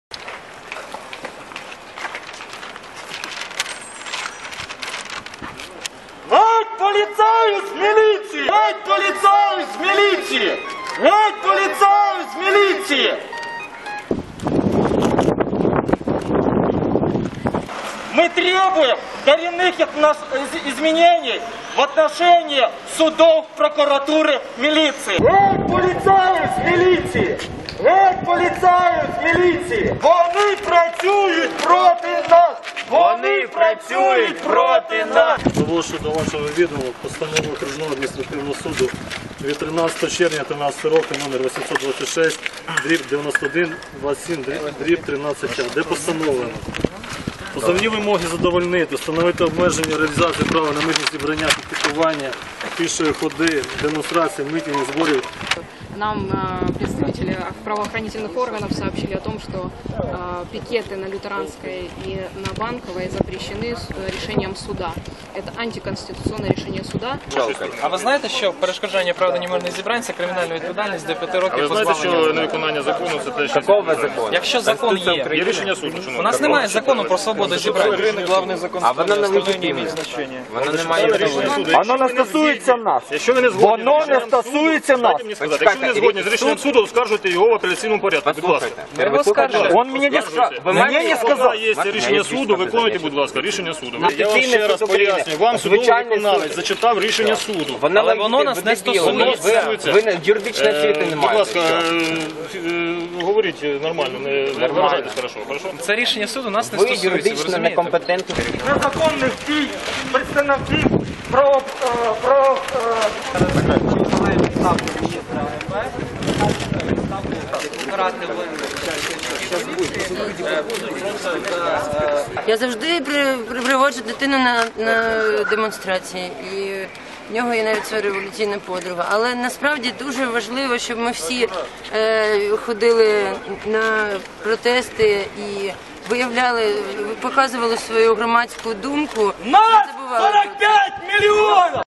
Активісти: Геть поліцаїв з міліції!
На Банковій під стінами Адміністрації Президента громадські активісти підтримали мешканців Врадіївки та вимагали права на мирний протест.